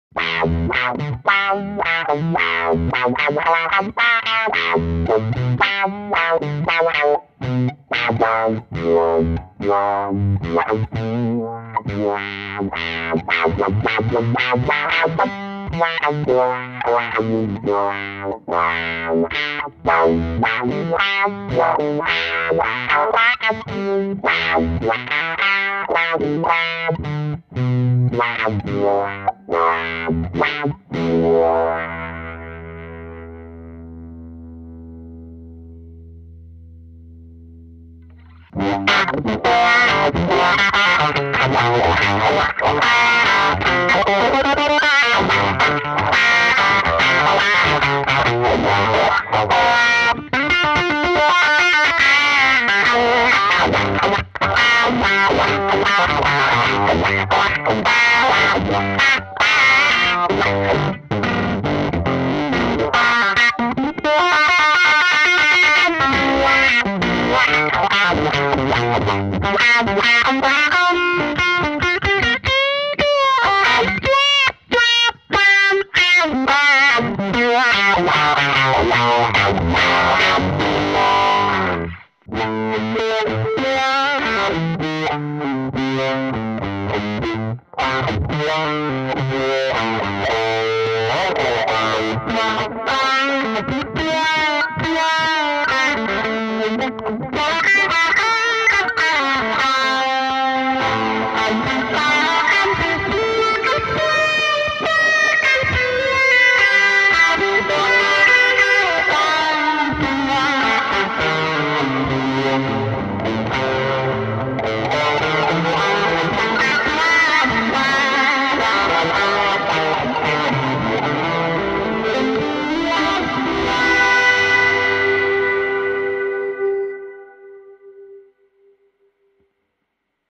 Reuss Silver Balloon wah
Reuss Silver Balloon:
A wah like no other, based on the vintage 1970s DeArmond Thunderbolt

Demoer: Nenad Patkovic